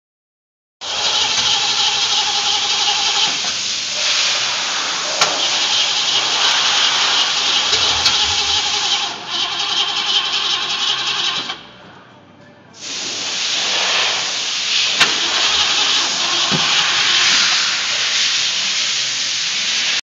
1977 Mustang II Cobra II ignition 1
Attempting to start my 1977 mustang with its new engine.
The distributor was one tooth off; thus, the fire.